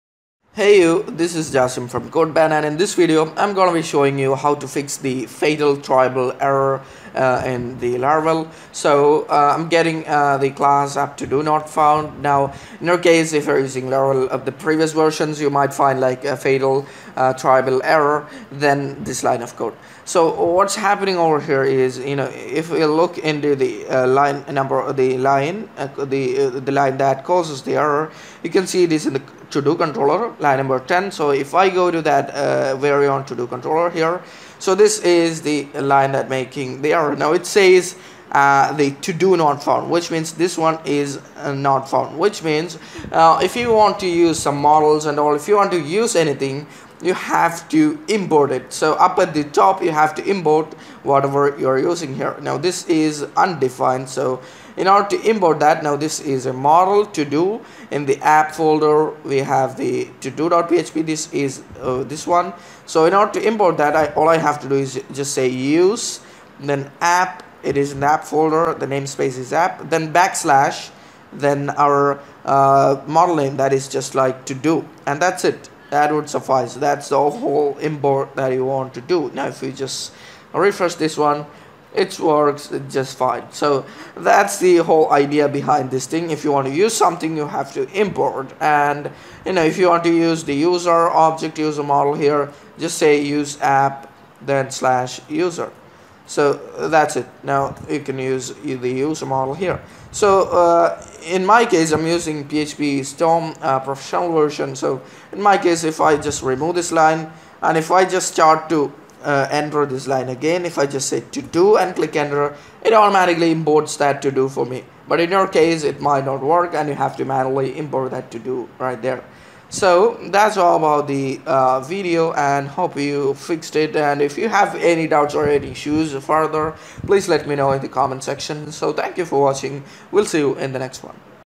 FatalThrowableError | Class not Found Error in Laravel | Solution
In this video, I'm gonna be showing you how to fix FatalThrowableError Class not Found Error in Laravel. Stay with us.
~Code Band